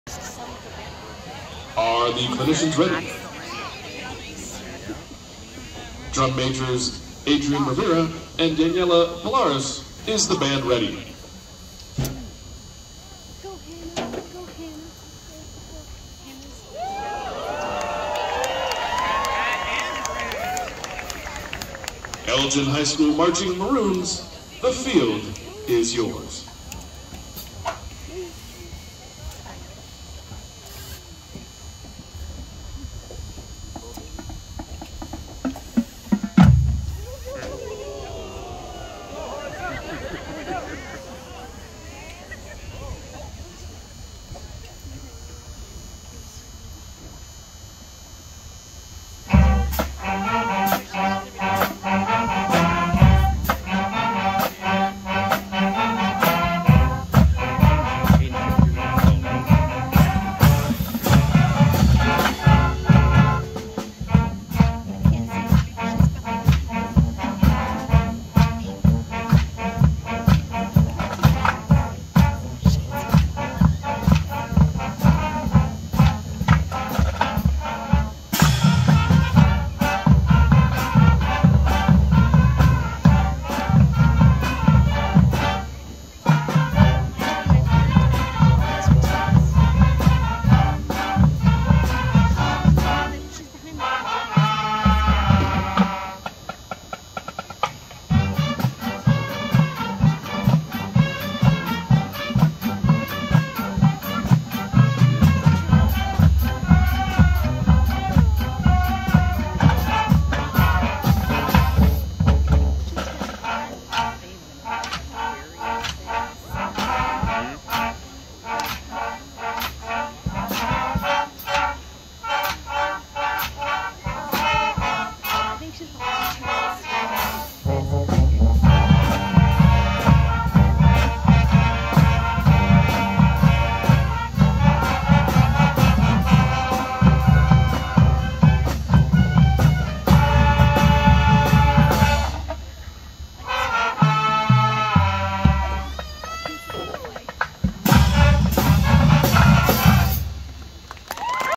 EHS Performance at U-46 Band Showcase - part 1 (2024)
Elgin (IL) Band and Color Guard performance during the Band Showcase event at Streamwood High School